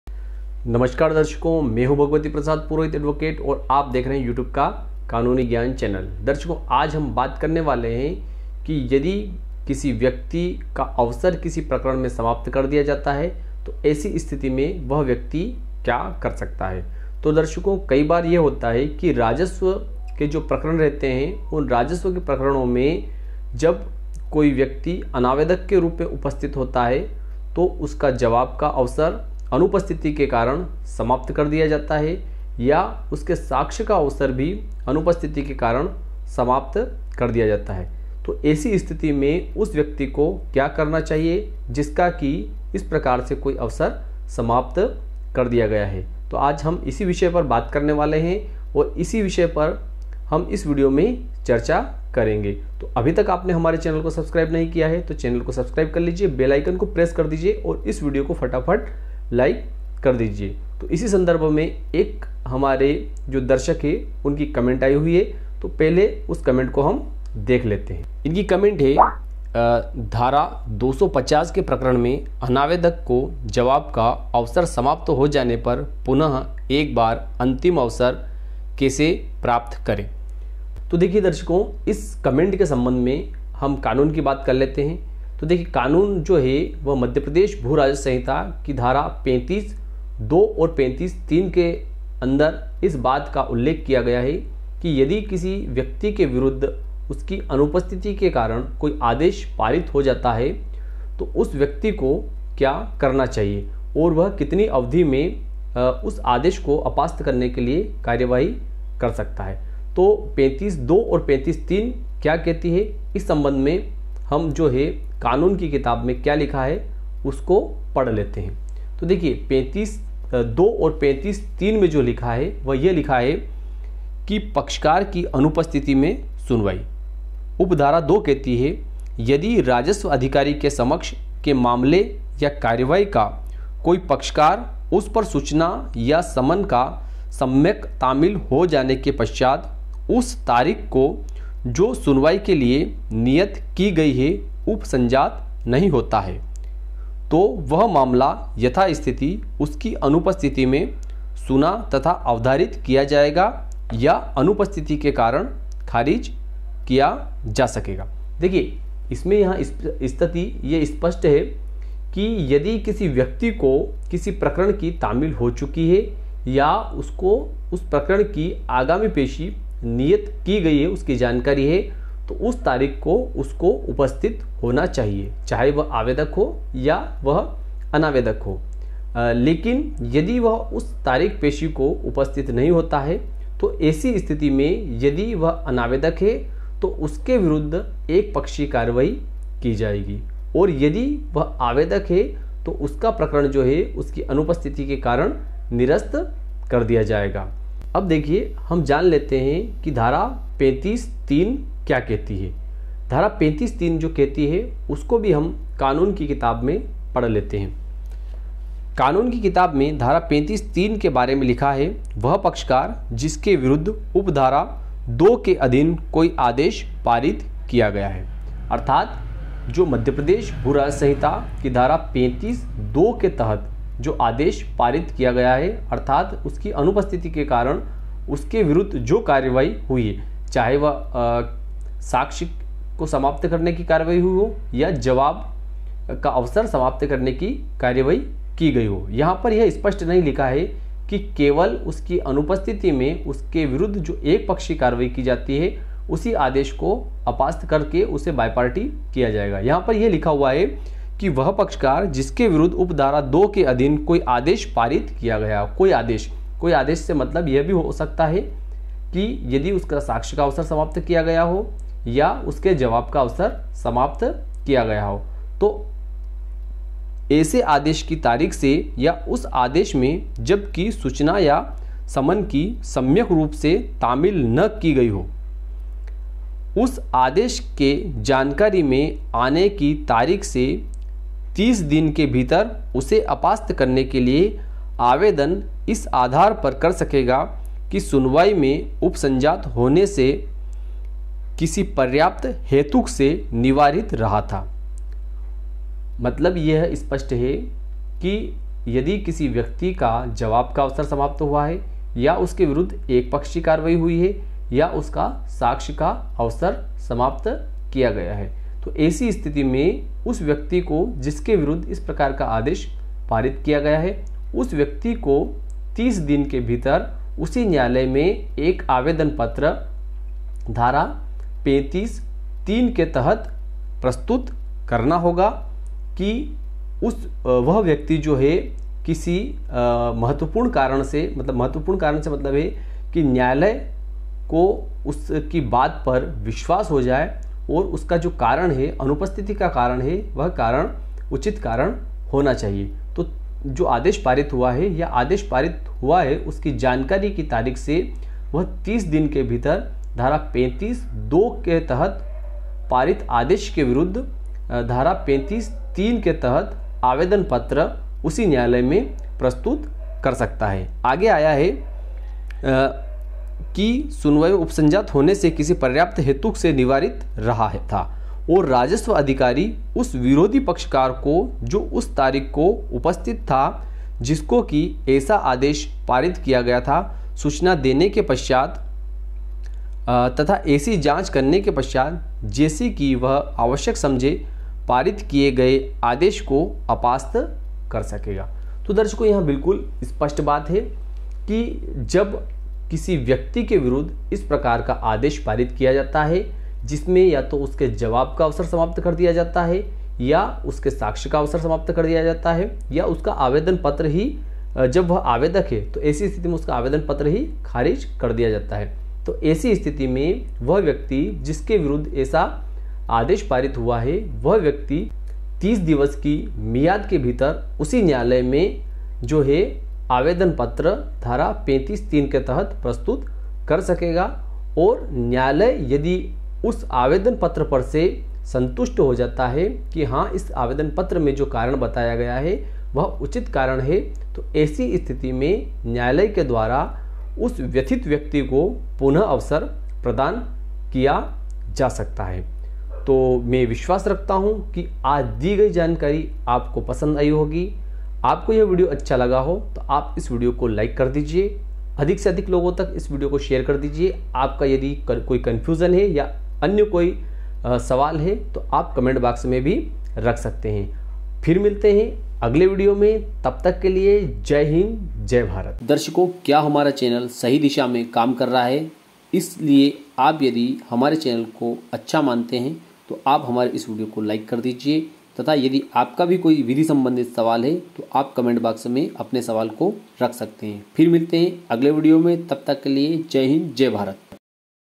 न्यायालय द्वारा अवसर समाप्त। वापस अवसर कैसे मिलेगा।
Cover Topic:-
1. न्यायालय द्वारा अवसर समाप्त करने पर क्या करें।
2. न्यायालय से वापस अवसर कैसे लें।
3. धारा 35 (3) मप्र भू राजस्व संहिता।
4. न्यायालय में अवसर समाप्त होने पर क्या कार्यवाही करें।
5. राजस्व न्यायालय में अवसर समाप्ति बाद क्या करें।
6. न्यायालय में क्या कार्यवाही करें कि फिर से अवसर मिल जाये।
7. अनुपस्थिति में अवसर समाप्त क्या करें।

Live :- Mahidpur ( महिदपुर )  District Ujjain Madhya Pradesh
Home :- Rabdaniya Ujjain M.P.